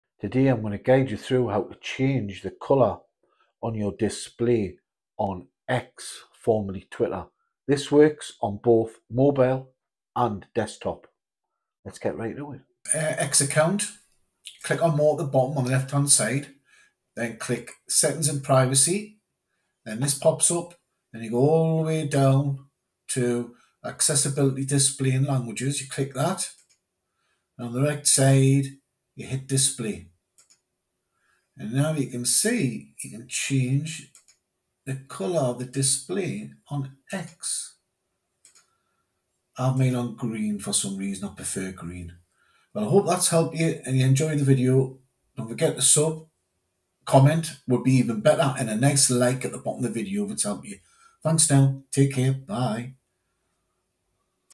How to Change the Display Color on X
I learn you how to change your display color on X, formerly Twitter, in this quick and easy tutorial. Personalize your experience with these simple steps!